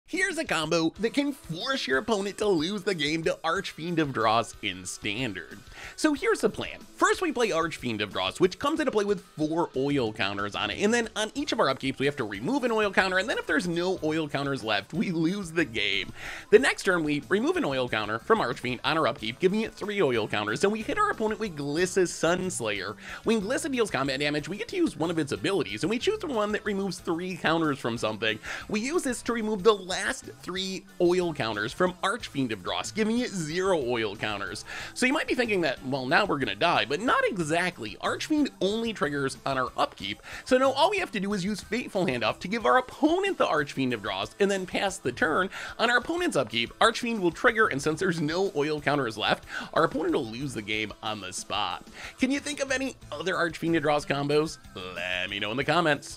Make Your Opponent Lose with Archfiend of the Dross in Standard! | MTG
Here's a combo that can force your opponent to lose the game with Archfiend of the Dross in Standard!